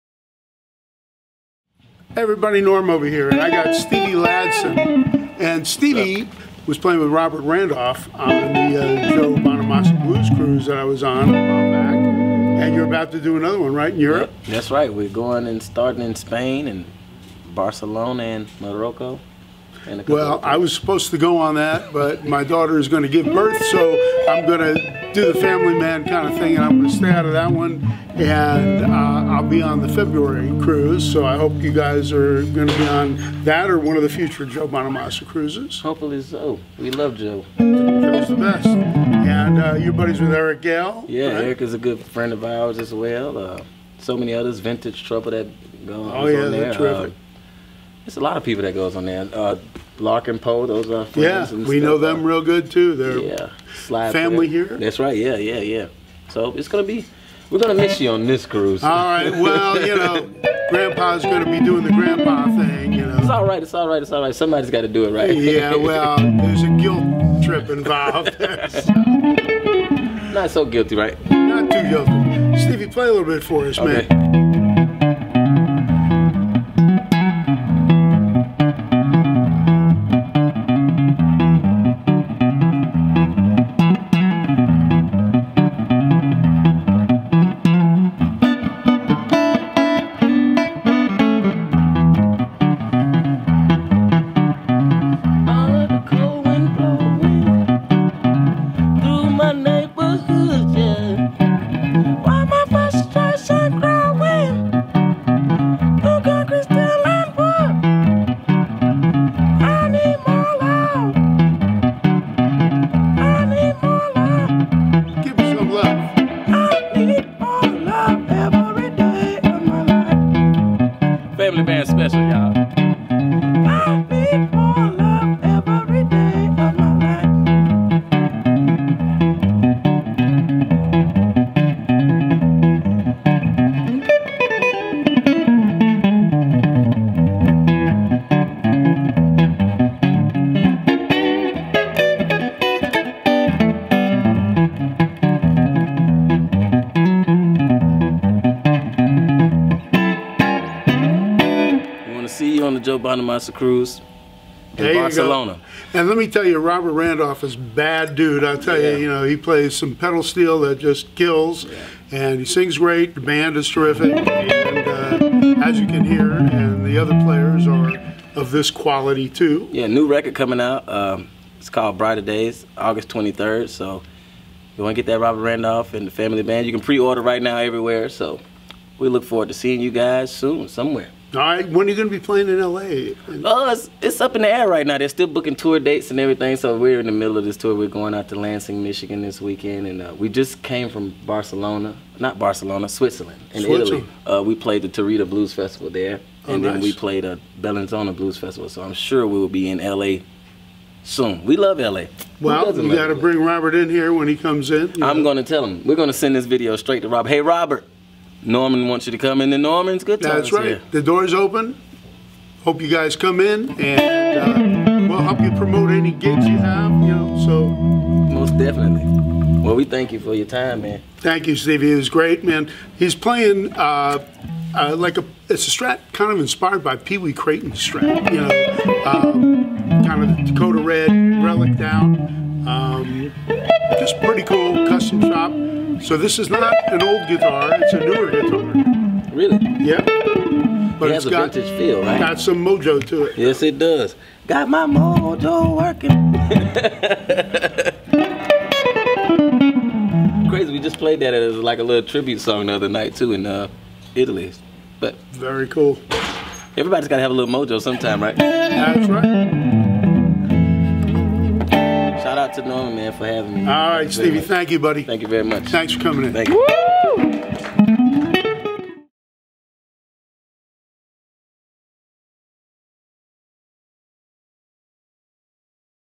Steve Ladson playing a Fender Custom Shop 1954 Stratocaster | Norman's Rare Guitars
Straturday is in full effect with this amazing guitarist! Check out Steve Ladson playing a Fender Custom Shop 1954 Stratocaster in Dakota Red and Gold here at NormansRareGuitars! What do you think?

MORE GUITAR VIDEOS/LESSONS/CONCERTS/ETC.